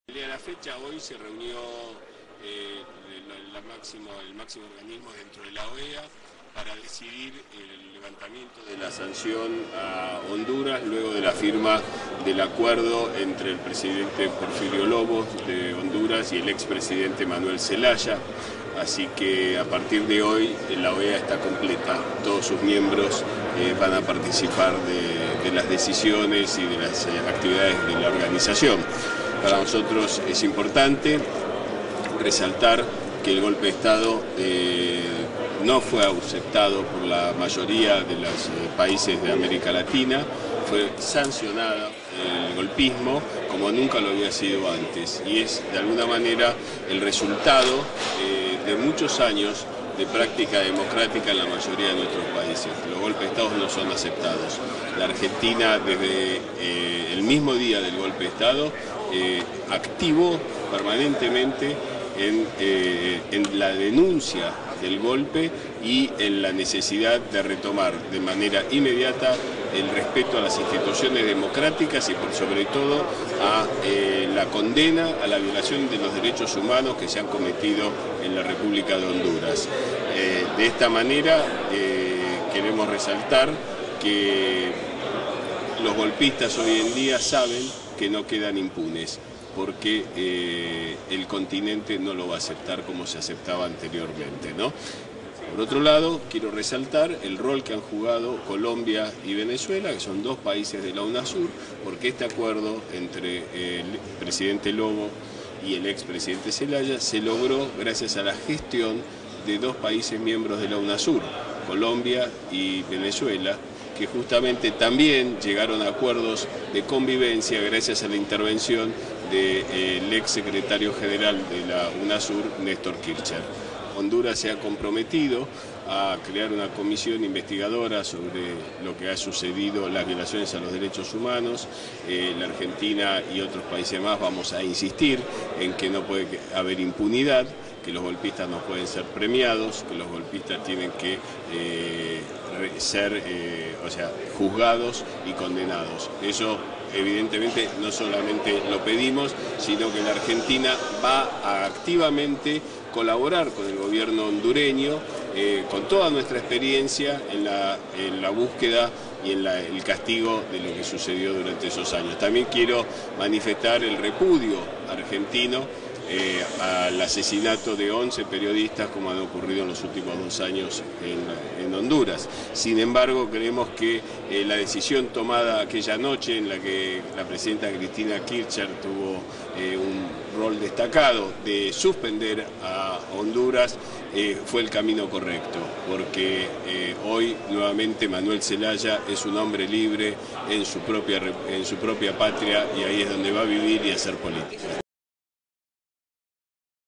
Timerman ratifica apoyo argentino al reintegro de Honduras a la OEA
El canciller, Héctor Timerman, ratificó en Roma el apoyo argentino al reingreso de Honduras a la OEA, tras el levantamiento de las sanciones a ese país luego del golpe de Estado que derrocó al ex presidente Manuel Zelaya.

    Timerman subrayó que aquel golpe de Estado no fue aceptado por los países de la región y fue sancionado, siendo Argentina una de las denunciantes que terminó con la suspensión de Honduras de la OEA.

    El canciller agregó que "Argentina activó aquella denuncia contra Honduras por el golpe de Estado" y subrayó que "en todo momento reclamó respeto a las instituciones democráticas y la condena a Honduras para demostrar que los golpistas no pueden quedar impunes".

    Timerman destacó también el rol de Colombia y Venezuela, países integrantes de la Unasur, para que la gestión de reconcer a Zelaya tuviera un buen término, y en especial en canciller recordó "las acciones llevadas a cabo por el ex secretario general de la Unasur, Néstor Kirchner".

    En otro orden, el canciller dijo que con la aceptación del retorno de Honduras a la OEA "se cierra aquel desgraciado capítulo, aunque habrá que crear una comisión investigadora, a la que el gobierno de Honduras se comprometió para que los golpistas sea juzgados y sancionados".

    Por último Timerman anunció que "el gobierno argentino va a colaborar con el gobierno de Honduras para llevar adelante esa comisión investigadora", al tiempo que señaló que "quiero aprovechar esta oportunidad para expresar el repudio argentino por los asesinatos de once periodistas en los últimos dos años".